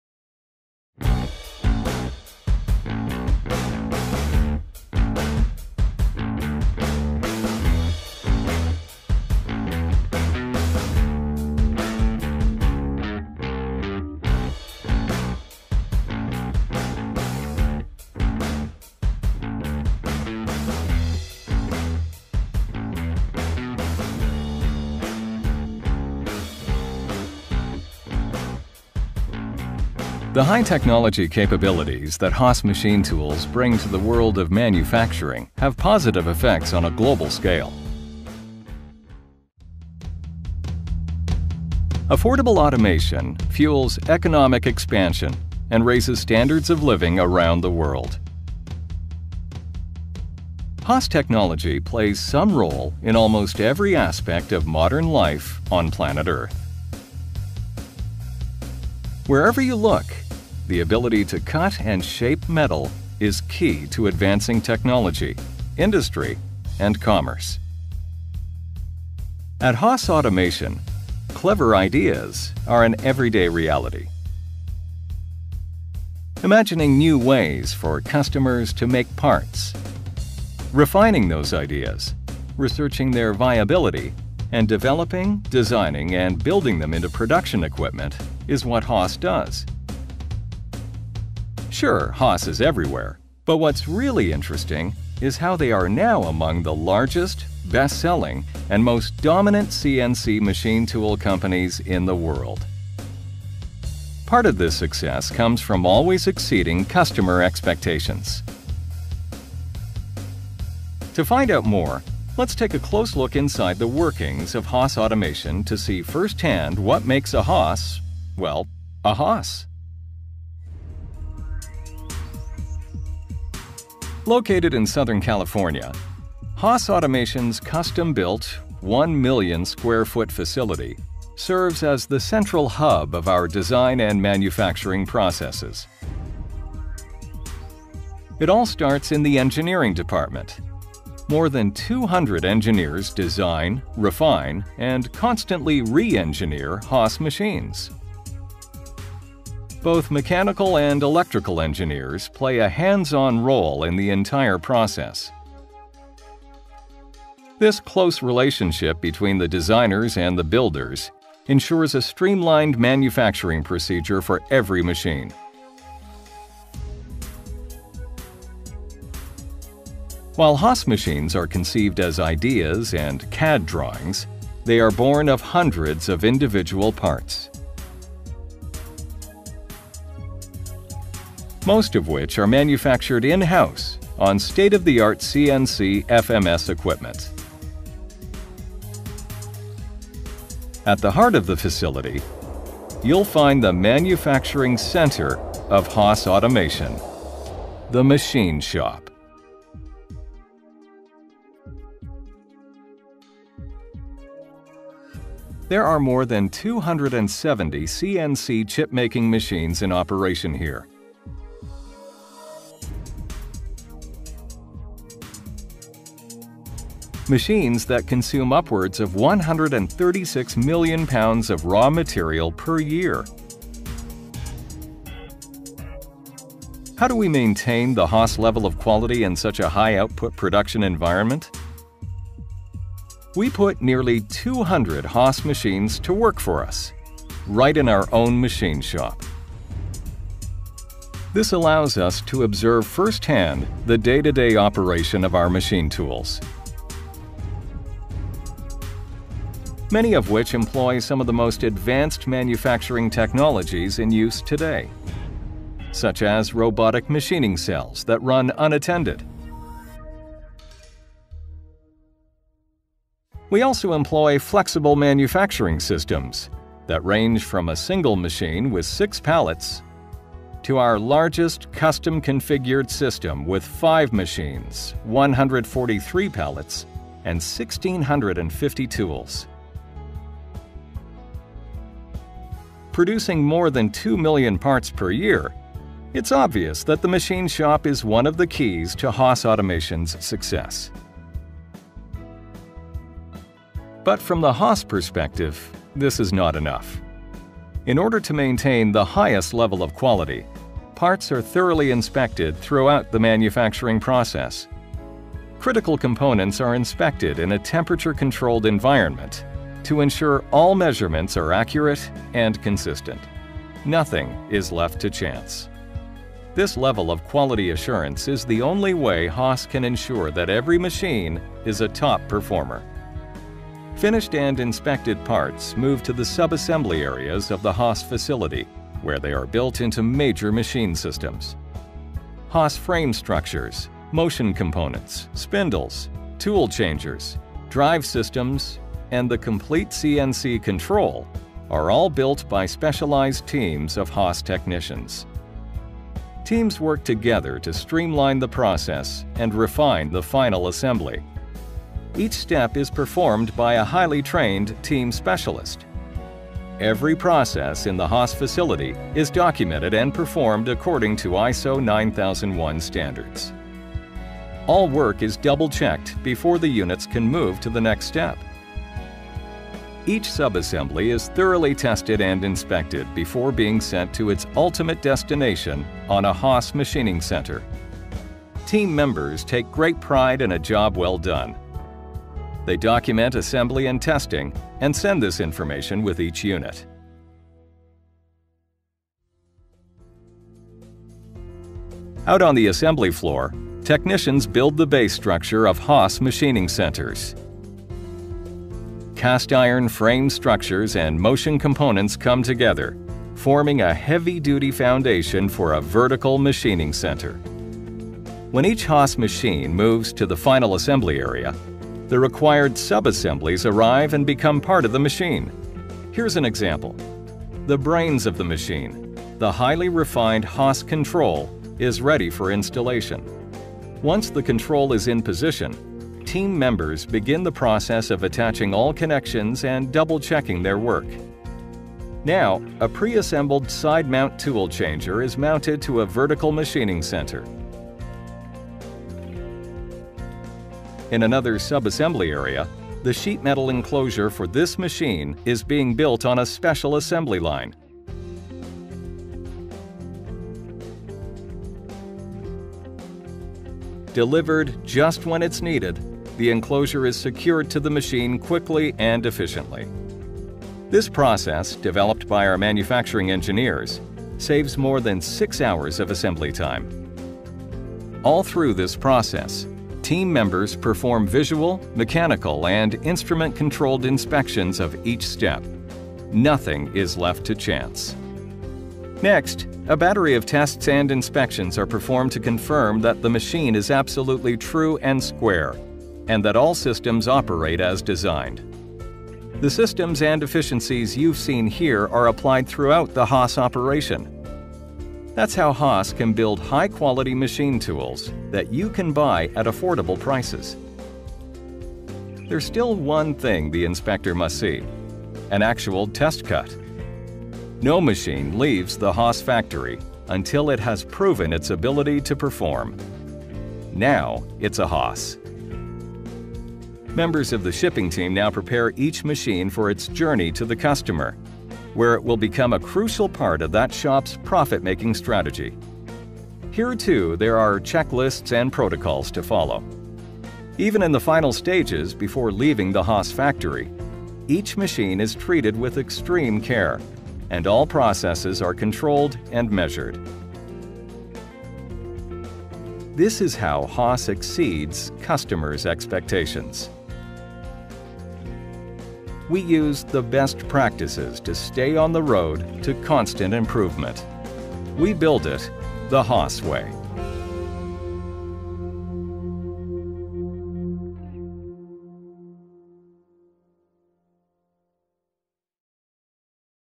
Dây chuyền sản xuất máy gia công CNC Haas
Chỉ cần học để sử dụng máy CNC mà chúng ta đã thấy khó khăn rồi, vậy thì công nghệ gì ở đây nữa, các tập đoàn lớn có thể sản xuất các loại máy móc từ nhỏ tới lớn với đủ chức năng một cách khá đơn giản, bạn hãy xem máy CNC được sản xuất như thế nào để thấy  những khó khăn trong công việc hoặc tiếp cận công nghệ của bạn hiện giờ không có là gì cả nhé.